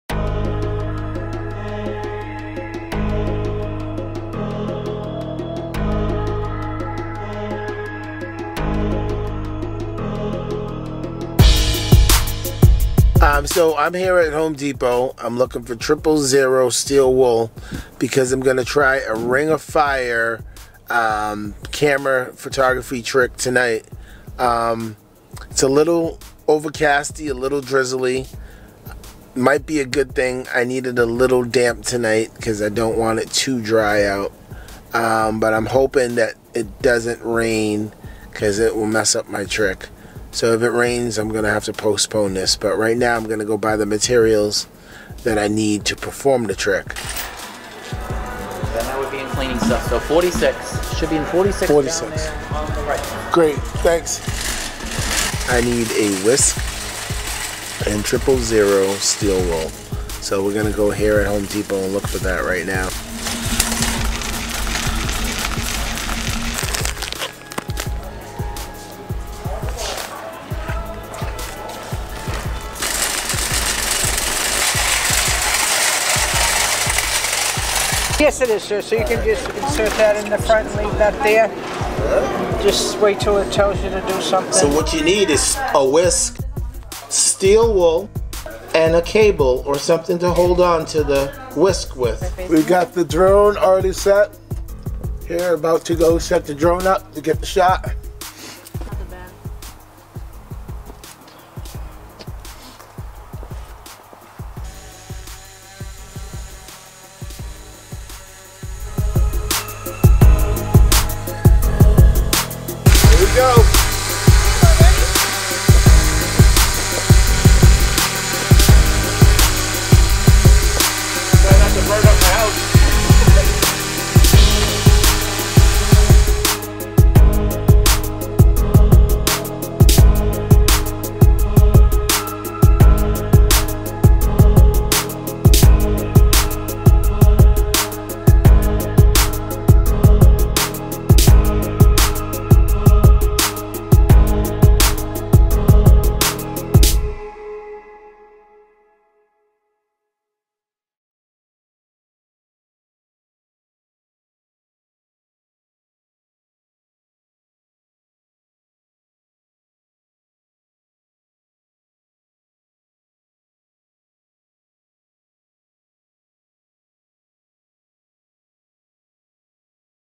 RING OF FIRE
This video is about Steel wool Fire
Join me as I try the burning steel wool photography trick.